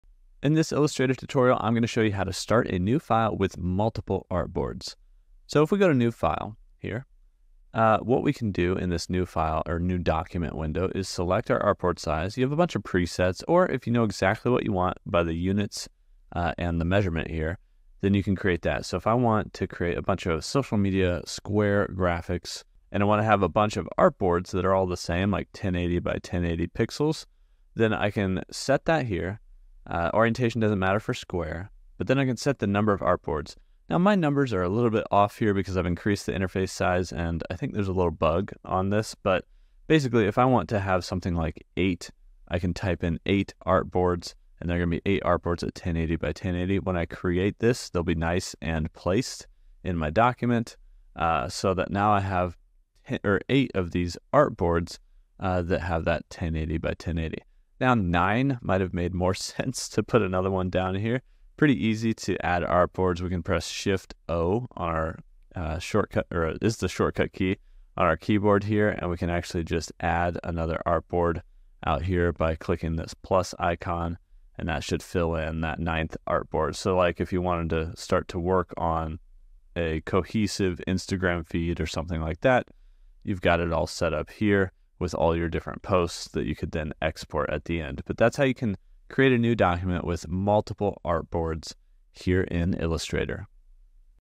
How to Make a New Document with Multiple Artboards in Illustrator
In this Illustrator tutorial, learn how to make a new document with multiple artboards in Illustrator!